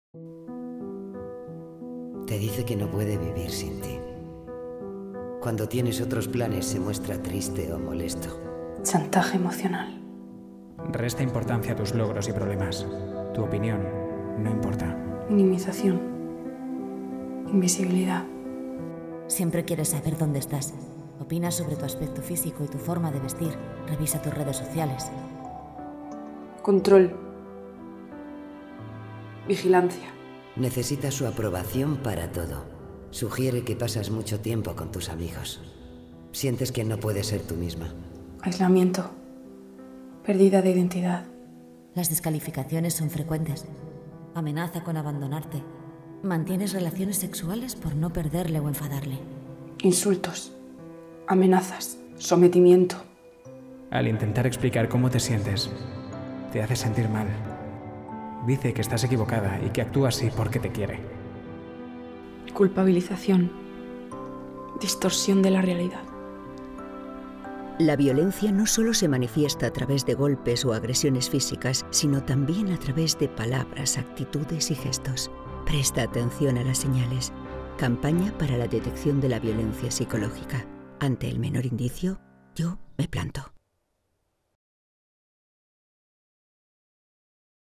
YO ME PLANTO - Campaña para la detección del maltrato psicológico
Campaña realizada para Biopic Films, para la prevención y detección de los maltrato psicológicos del Ayuntamiento de Elche.